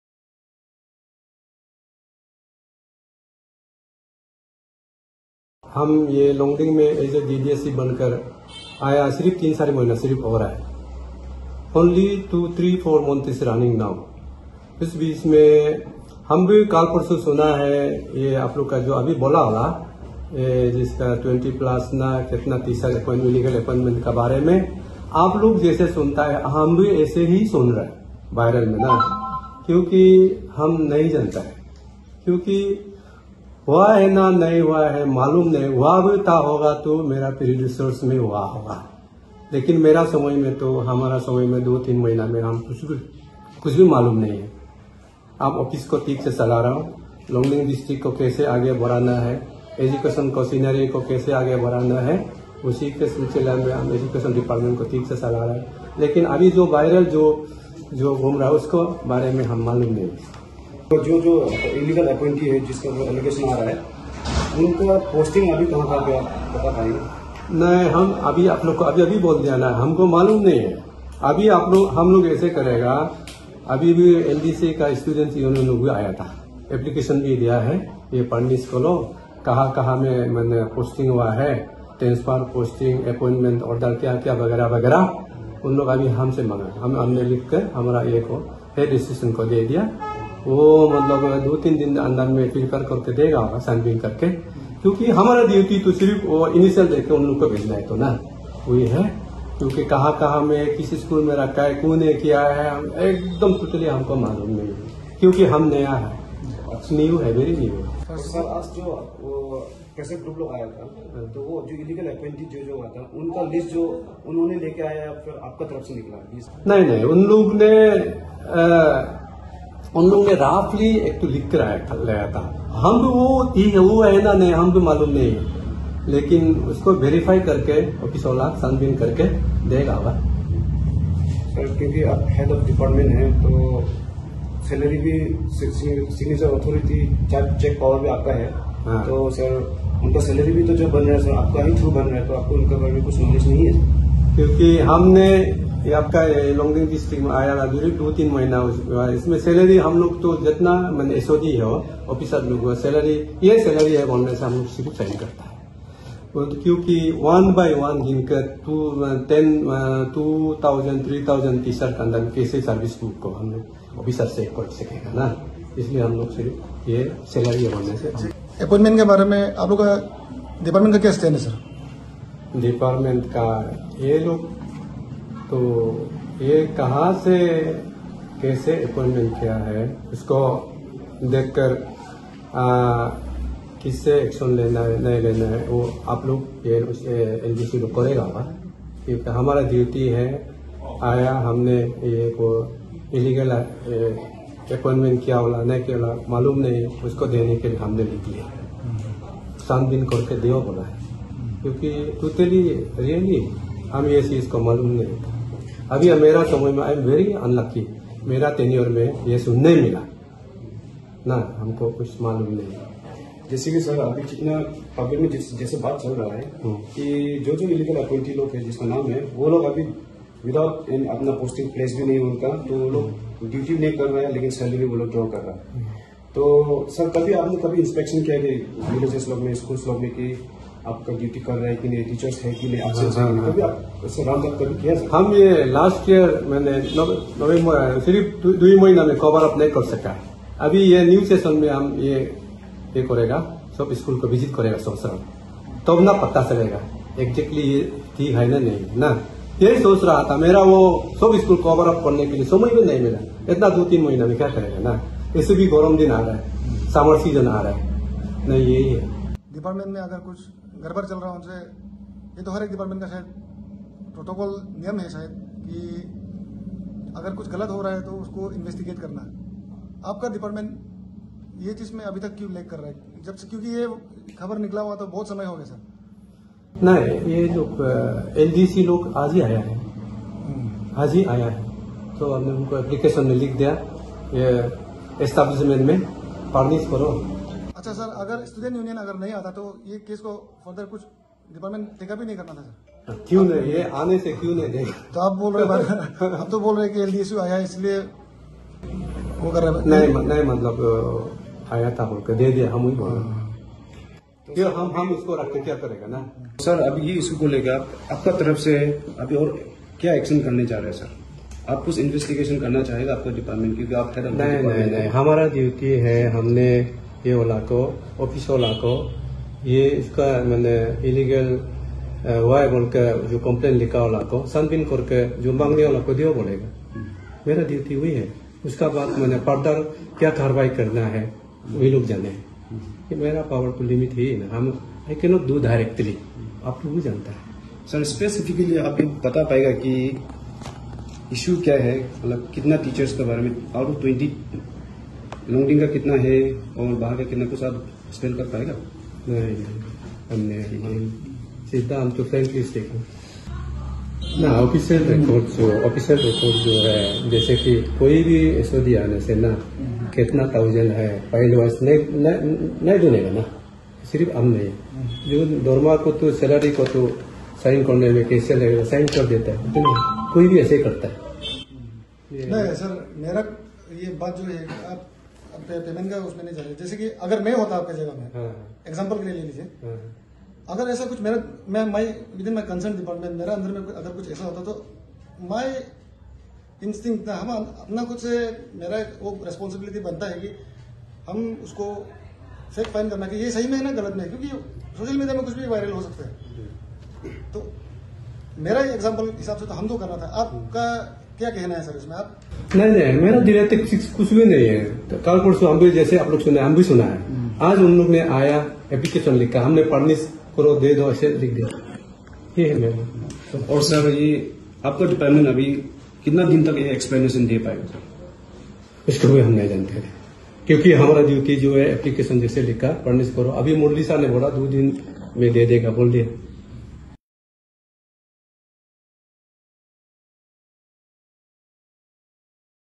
DDSE, Longding clarifies on alleged illegal appointment of Teachers
More than twenty teachers appointed through backdoor by illegal means," alleged the all Longding district students'union